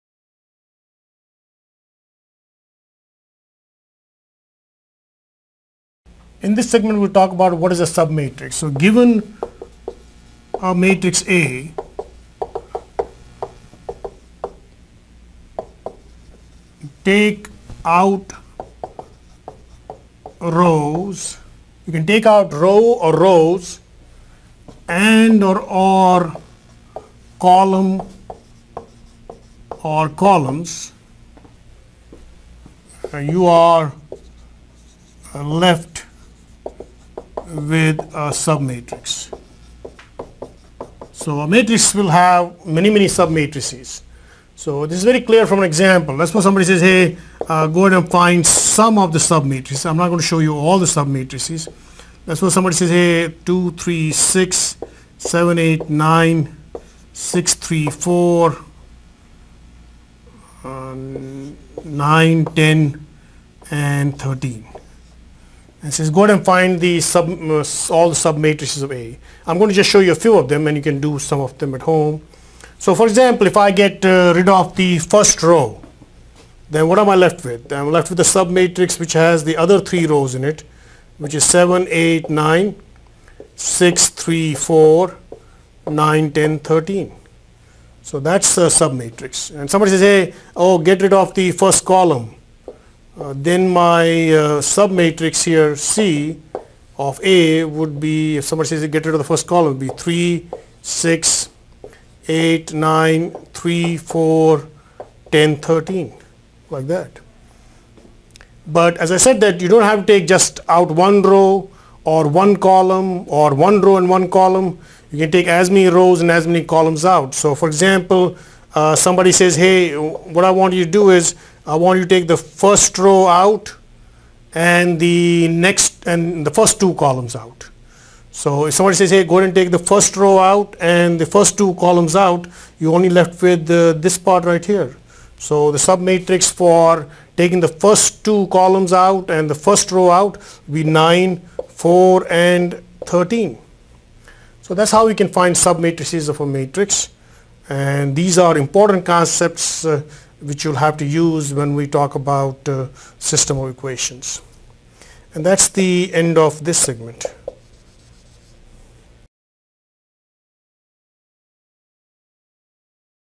Chapter 04.01: Lesson: What is a submatrix?
Learn via an example what is a submatrix. For more videos and resources on this topic, please